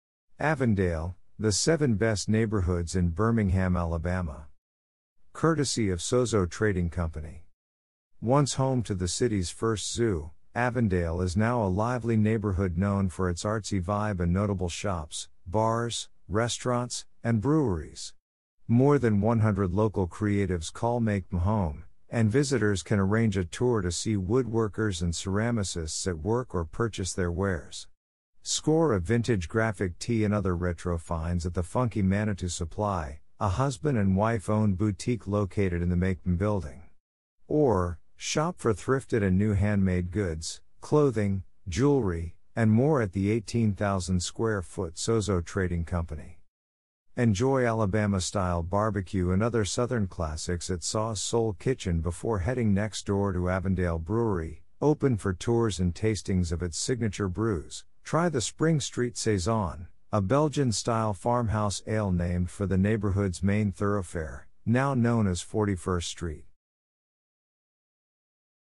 Avondale
Avondale - An episode of series: The 7 Best Neighborhoods in Birmingham Alabama.
Courtesy of Sozo Trading Co..
Once home to the city's first zoo, Avondale is now a lively neighborhood known for its artsy vibe and notable shops, bars, restaurants, and breweries. More than 100 local creatives call MAKEbhm home, and visitors can arrange a tour to see woodworkers and ceramicists at work or purchase their wares. Score a vintage graphic tee and other retro finds at the funky Manitou Supply, a husband-and-wife-owned boutique located in the MAKEbhm building. Or, shop for thrifted and new handmade goods, clothing, jewelry, and more at the 18,000-square-foot Sozo Trading Co. .
Enjoy Alabama-style barbecue and other Southern classics at Saw's Soul Kitchen before heading next door to Avondale Brewery, open for tours and tastings of its signature brews; try the Spring Street Saison, a Belgian-style farmhouse ale named for the neighborhood's main thoroughfare, now known as 41st Street..